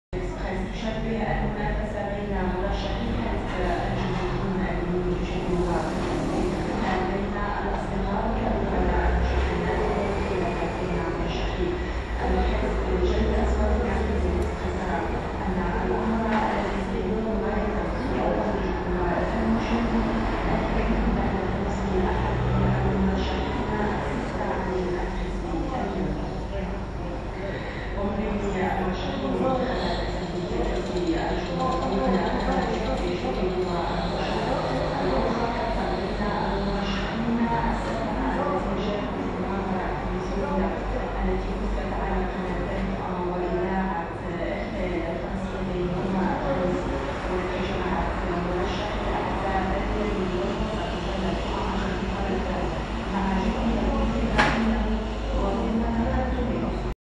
A Chinese worker acrobat panic canvas !!
A Chinese worker acrobat panic canvas !! Since 08 October, Shi Shenwei yards young worker.Un ouvrier chinois, acrobate affole la toile !! Depuis le 08 octobre, Shi Shenwei jeune ouvrier de chantiers Un ouvrier chinois, acrobate affole la toile ! ... Depuis le 08 octobre, Shi Shenwei jeune ouvrier .A Chinese worker acrobat panic canvas! ... Since October 8, Shi Shenwei young worker